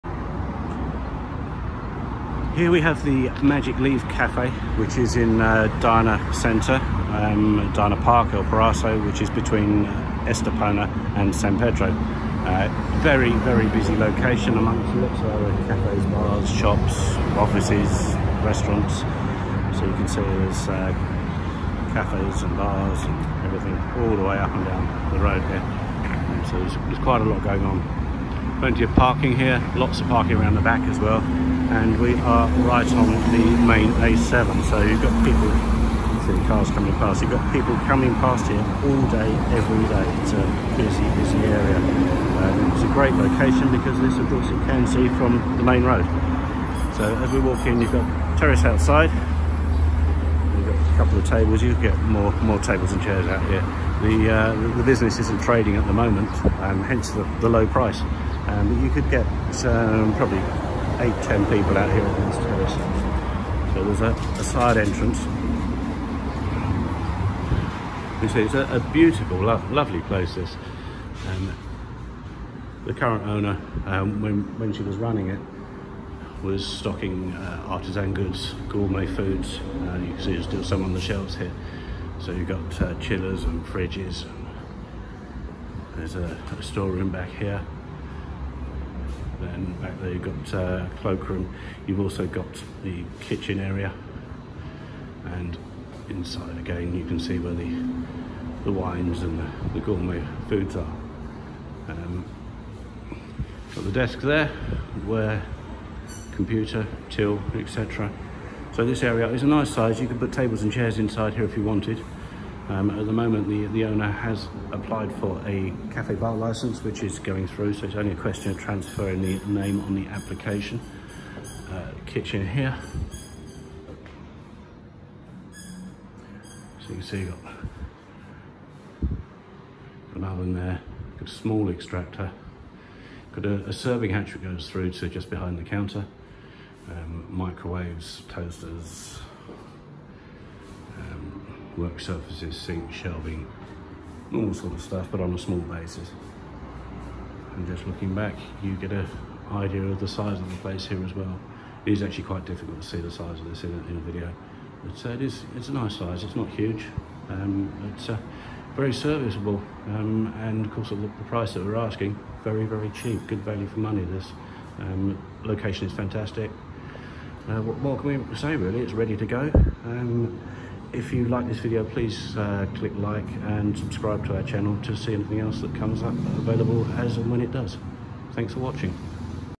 Estepona Cafe / Gourmet Shop for Traspass (lease for sale) BARGAIN - 22.500€ plus rent of 800€ pm.
This café is located in very busy commercial centre, it is visible from the main A7 and people walk or drive past all day.

The owner needs to sell quickly and has reduced it from 35.000€ to a BARGAIN PRICE of 22.500€.

It was renovated around two years ago and has a modern rustic feel. Currently the owner concentrates on selling health food and drinks both to eat in and to take away. They also offer a number of gourmet items such as wines, oils and olives.

There is around 50m2 inside and 12m2 outside.

There will be a new 5 year lease which is subject to a rental of 800€ per month.

The Current owner has a S.L. (limited company), which can be included if required.